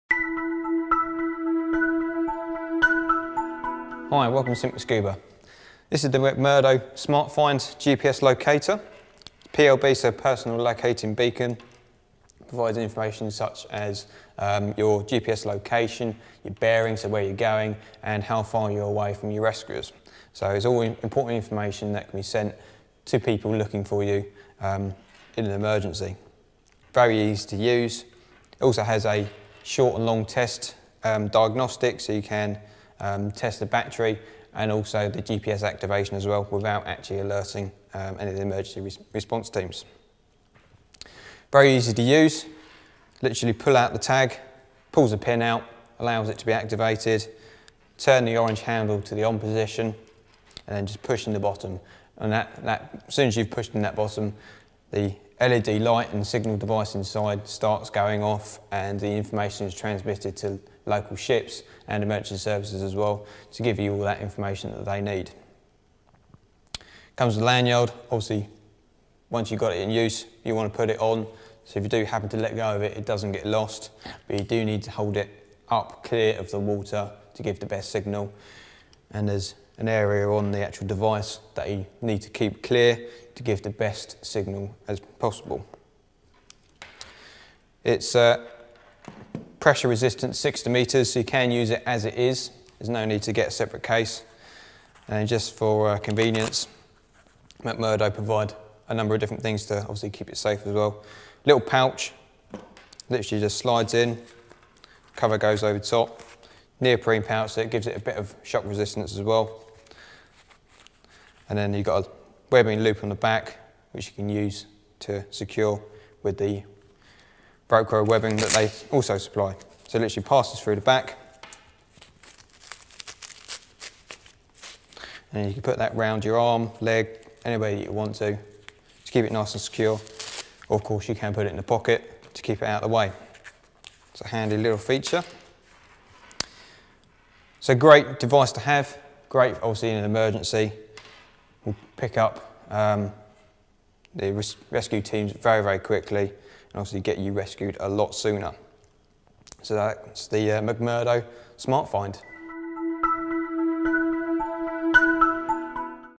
McMurdo Smartfind S10 AIS Beacon
The MucMurdo Smartfind S10 is a manually activated personal AIS (Automatic Identification System) Beacon that transmits structured alert messages, GPS position information and the devices unique identification number to all AIS enabled vessels and land based VTS stations within a typical four mile radius. When activated the beacon signals that help is needed in a man overboard or lost diver situation.

The Smartfind S10 is waterproof to 60m, will transmit continuously for at least 24 hours and has a five year battery storage life. The beacon has a built in flashing light for visual location in low light or night conditions.

Features
Manually activated personal safety device to aid recovery of persons in difficulty
Transmits GPS target tracking information over AIS
Unique serialized ID
Small and light for unobtrusive carriage
Waterproof, buoyant and fully submersible to 60 meters
Flashing LED light
Minimum 24 hour continuous operation
5 year battery storage life